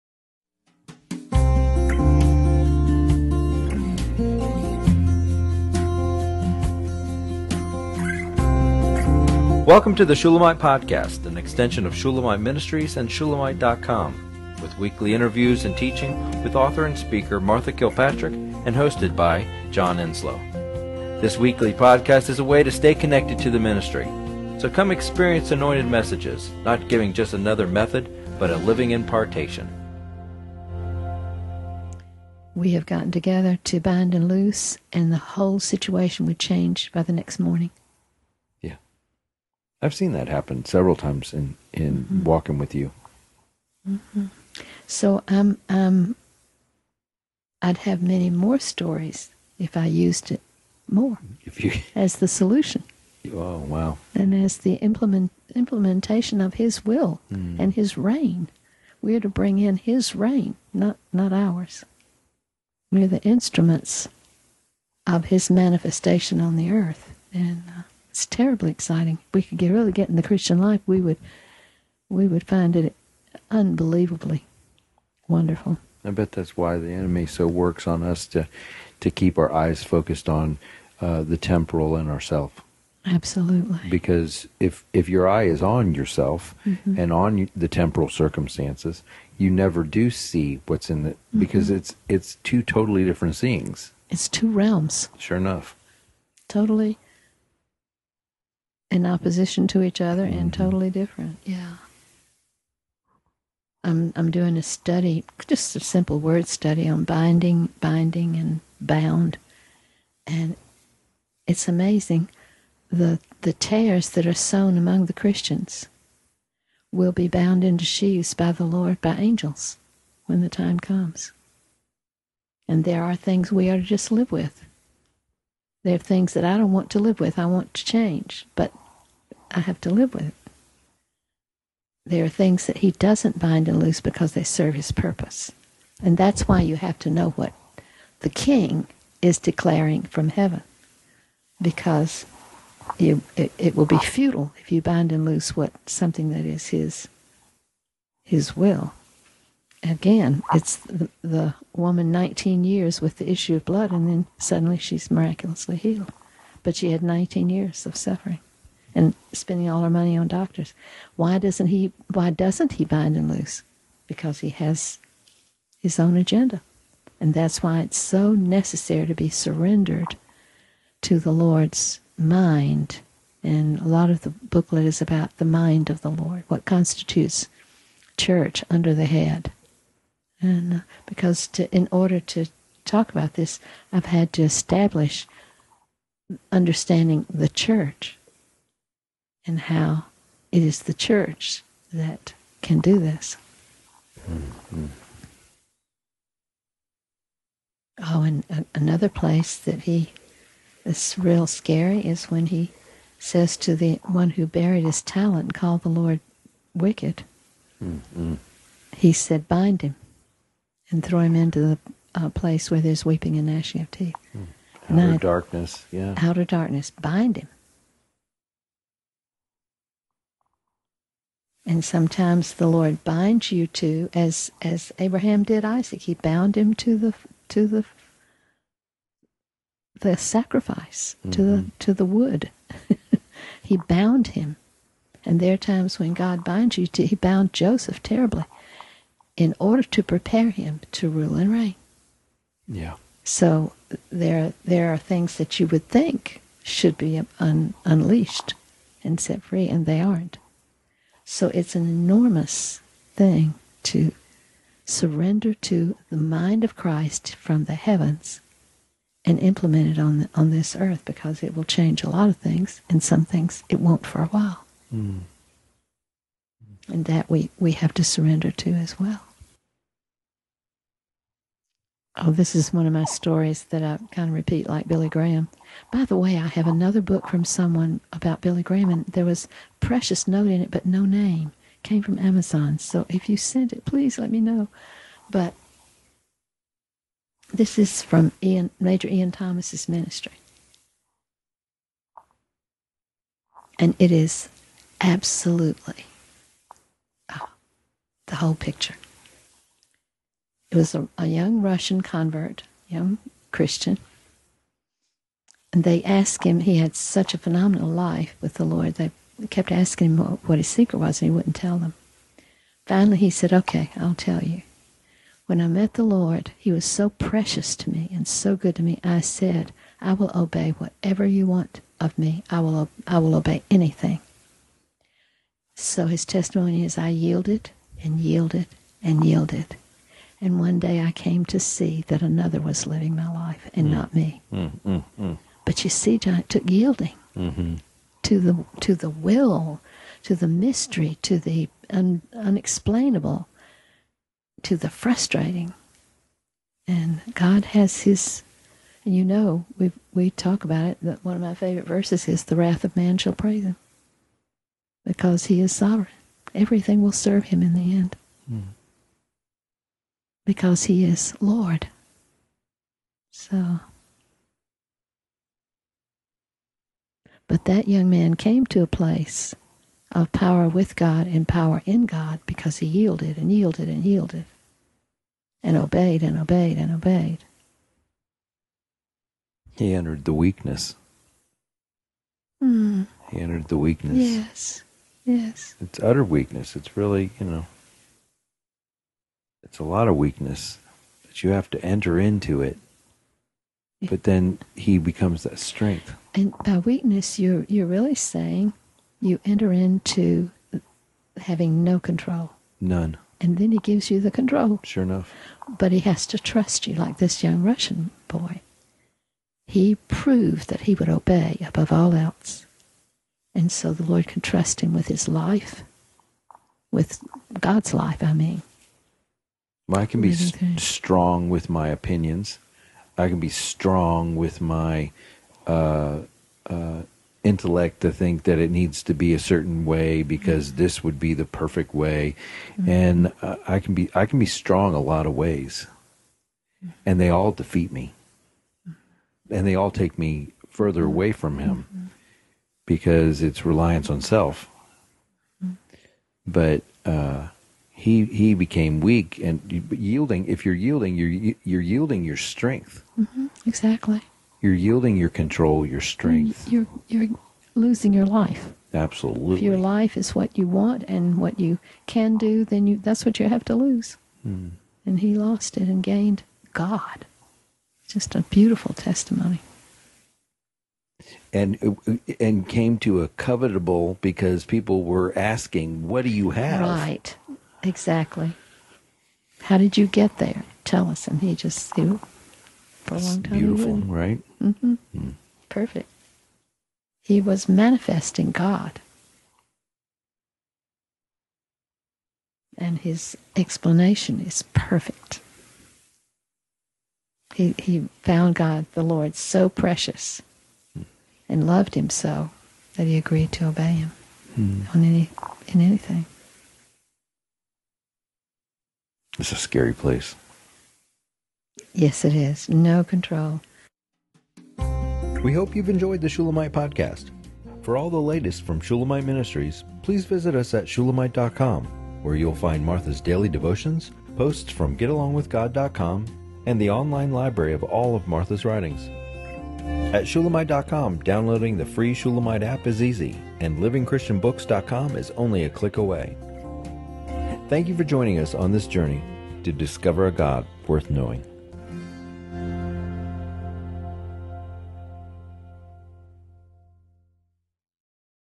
A Life That Will Bind and Loose – Episode #613
Do you look at warfare in this life, like the ability to bind and loose, as coming from a place of strength? Is it strength that you seek to fight the enemy

Remember the complete transcripts will always be on ShulamitePodcast.com. The transcription for A Life That Will Bind and Loose is found ShulamitePodcast 😁👍